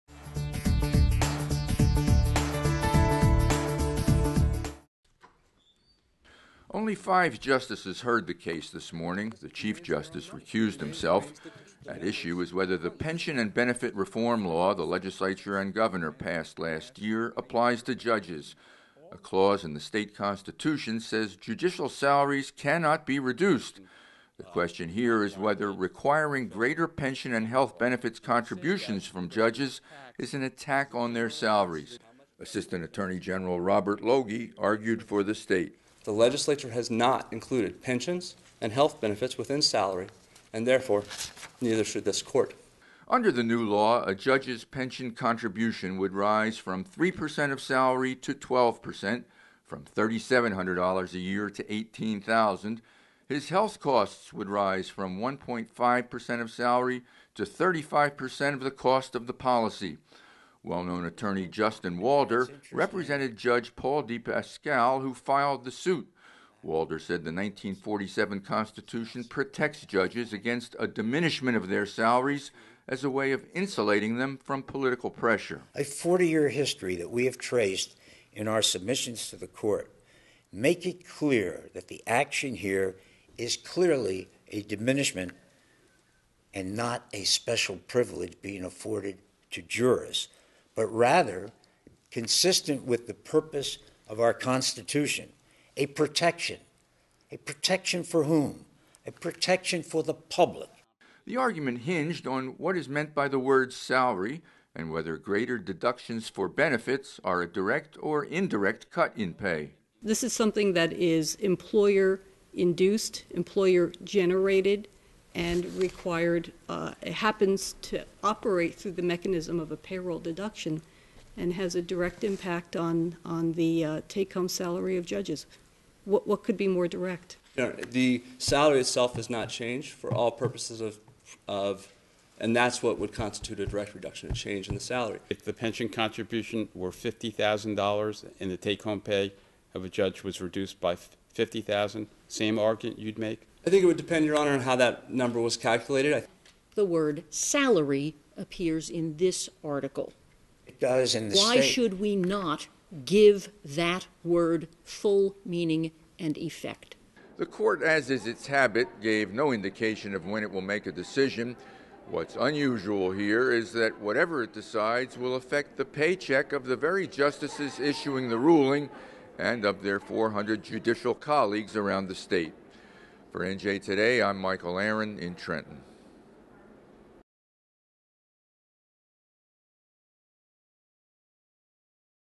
Supreme Court Deciding If Pension and Benefit Increase for Judges Is Legal
The New Jersey Supreme Court is hearing a case that will decide if the pension and benefit reform the Legislature and Gov. Chris Christie passed last year applies to judges. A clause in the state constitution says judicial salaries cannot be reduced. The state's representative argues that pension and health benefit contributions are separate from salaries and therefore legal, while representatives for judges say requiring increased contributions to pensions and health benefits reduces the take-home pay and therefore salaries of judges, making the practice illegal.